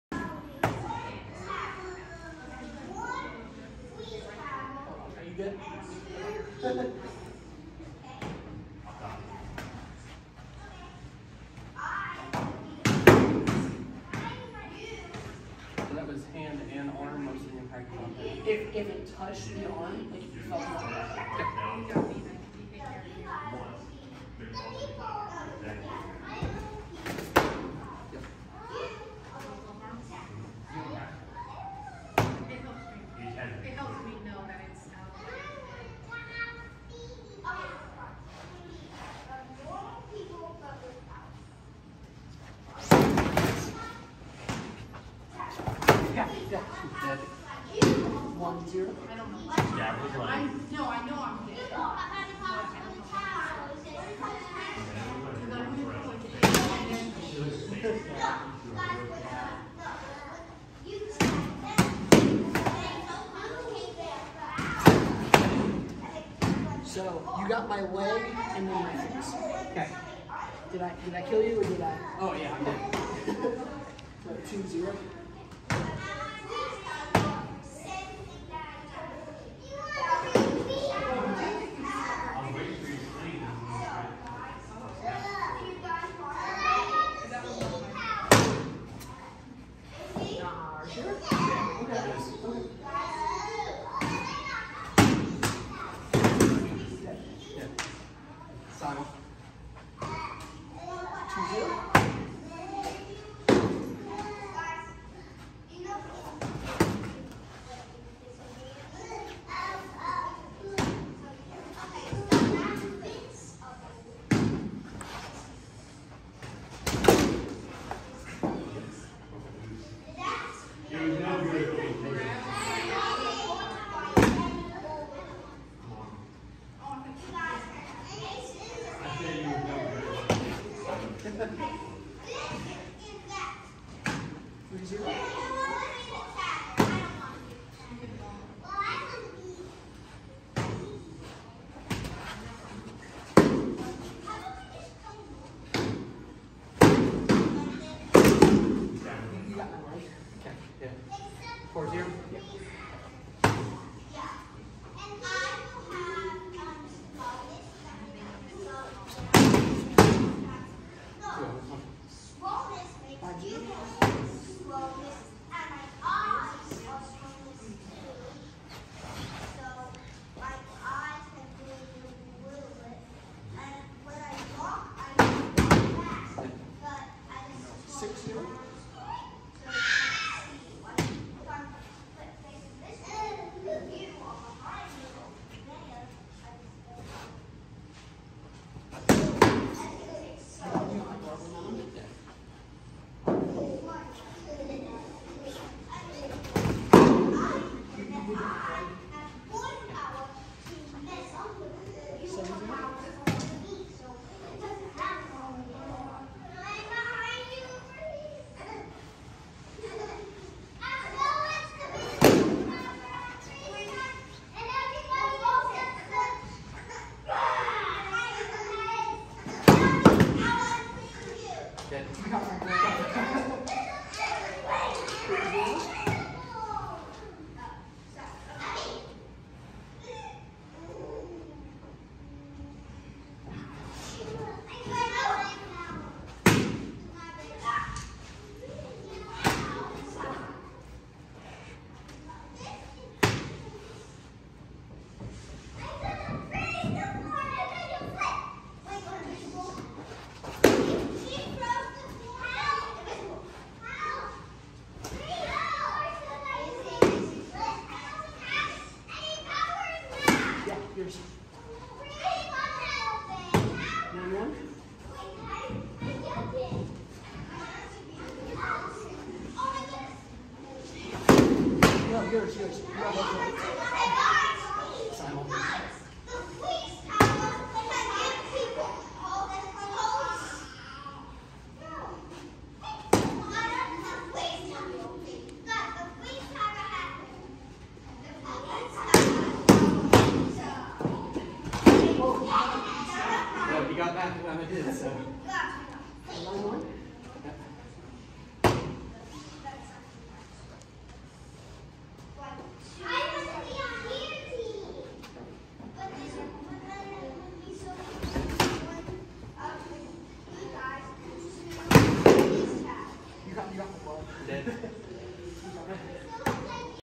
First To Ten: Godric vs Silent Knight | Two Sword | 12/22/2021
Hey All!

This is a drill called First to Ten. Madness and I fought until one of us had won 10 rounds total. This is a useful drill for larps like Amtgard, Dagorhir, Belegarth, Darkon and many others.